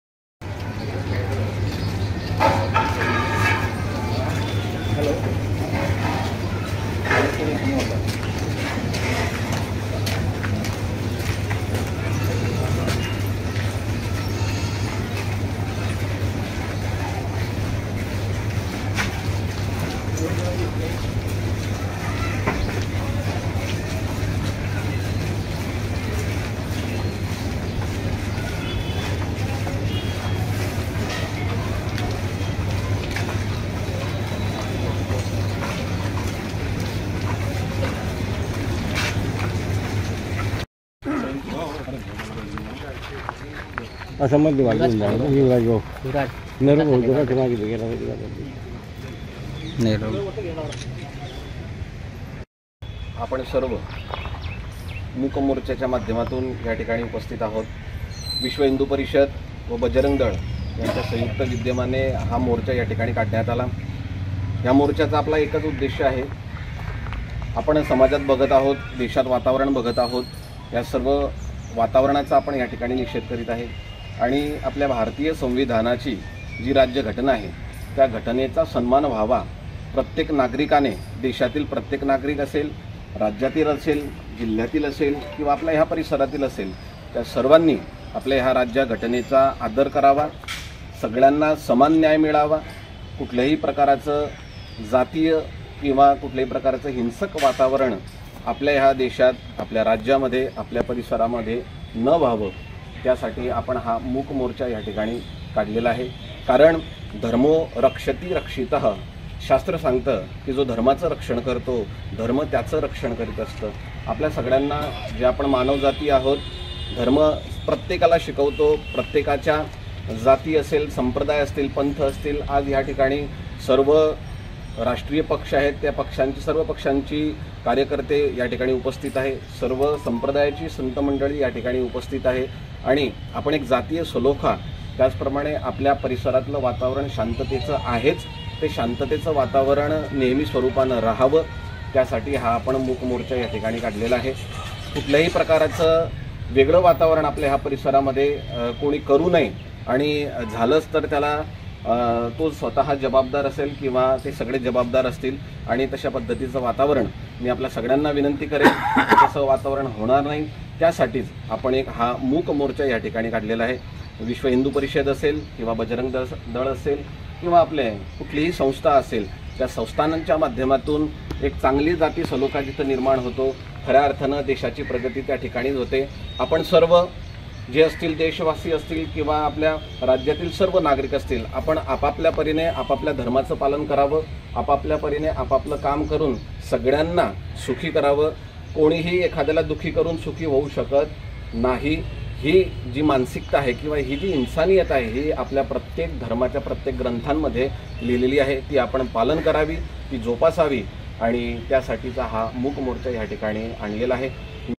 Faizpur : विश्‍व हिंदू परिषद व बजरंग दलाचा फैजपुरात निषेध मोर्चा
फैजपूर, ता. यावल-निलेश पाटील | देशभरात हिंदूंवर होत असलेल्या अत्याचारांचा निषेध करण्यासाठी आज शहरातून विश्‍व हिंदू परिषद आणि बजरंग दल यांच्यातर्फे मूक निषेध मोर्चा काढण्यात आला. यात स्वामी भक्ती किशोरदास शास्त्री, राम मनोहरदास, कन्हैया महाराज, अनंत प्रकाश शास्त्री आदी संत मंडळीसोबत नागरिक मोठ्या संख्येने सहभागी झाले. मोर्चानंतर प्रशासनाला निवेदन देण्यात आले. पहा याबाबतचा व्हिडीओ वृत्तांत.